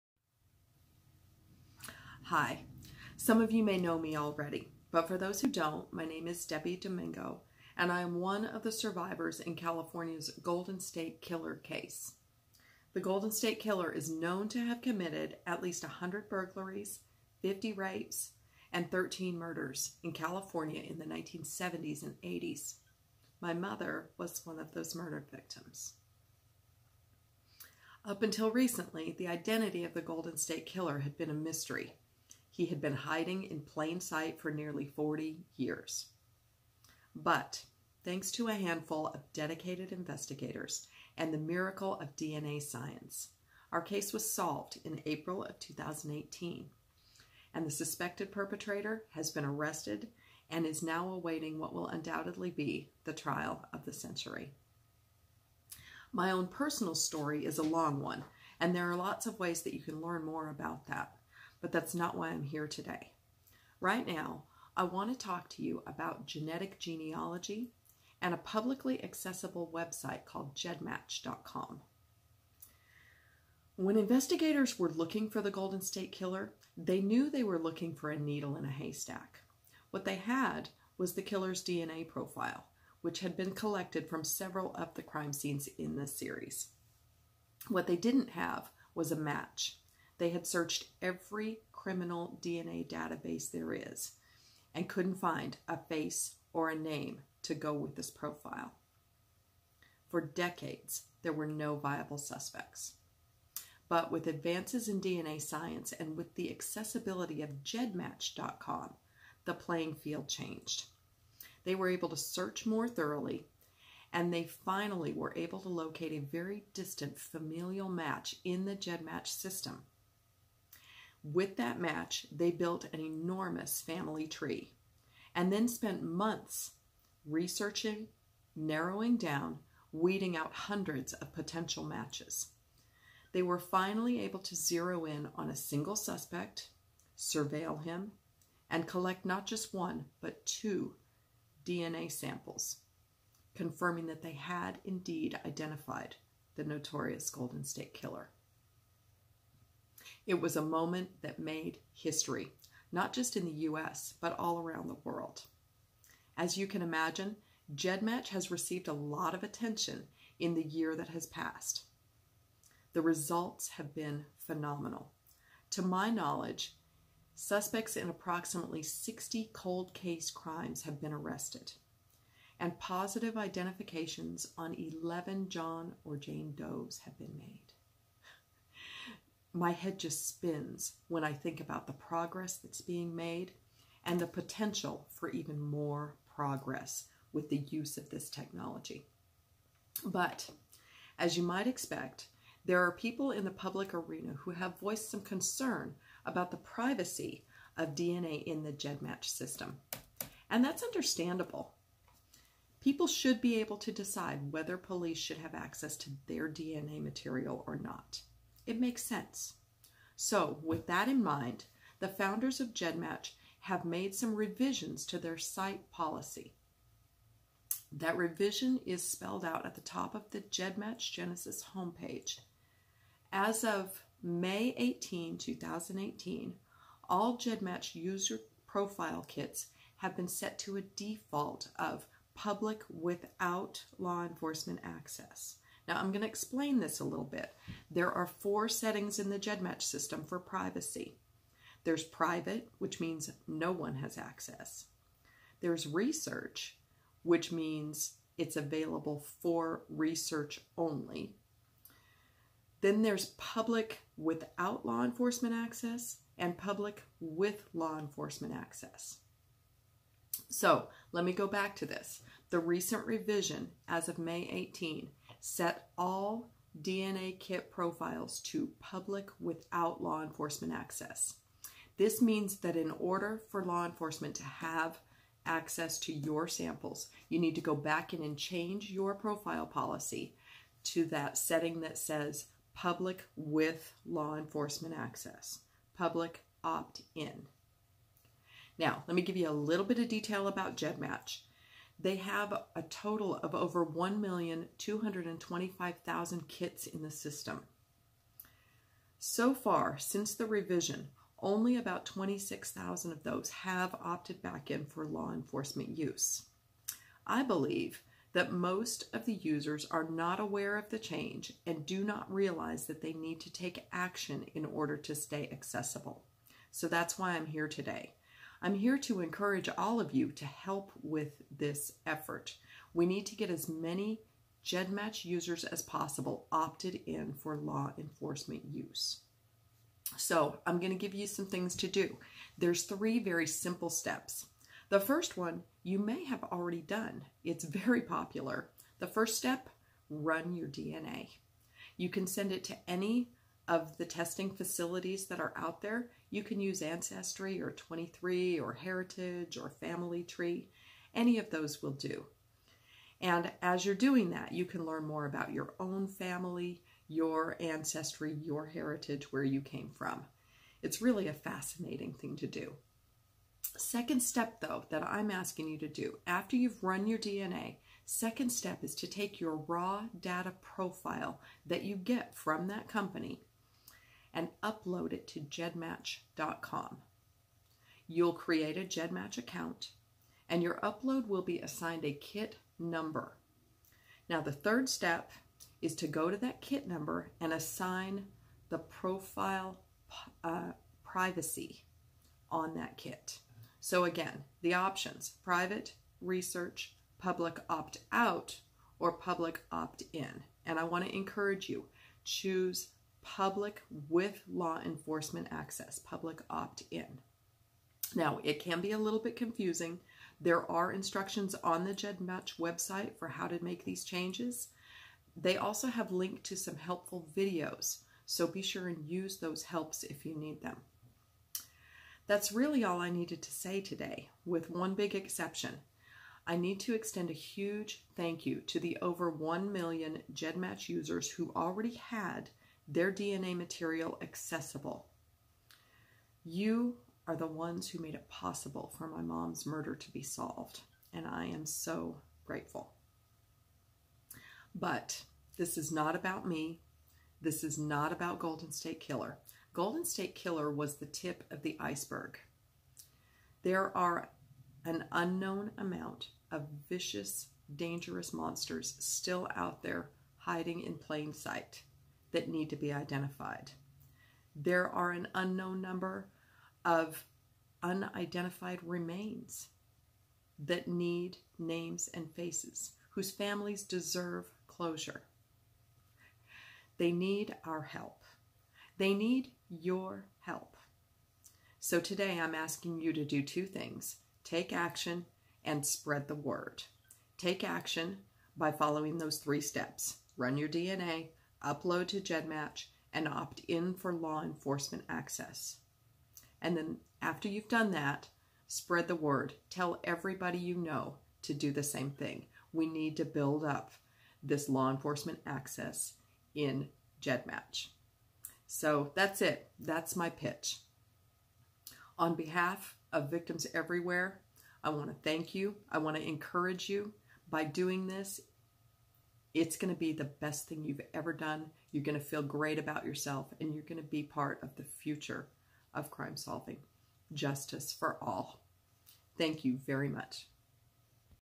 Upload your DNA on GEDmatch.com & Opt in!
The miracle of DNA science is doing INCREDIBLE things!
-Reconnecting families
-Narrowing down suspect pools
-Solving crimes (confirming the guilty, and exonerating the innocent!)
-Identifying the remains of "John & Jane Does"
But we NEED YOUR HELP! Have your DNA tested, Upload it to GEDmatch.com, and set your privacy to "Public WITH Law Enforcement Access."  Then spread the word!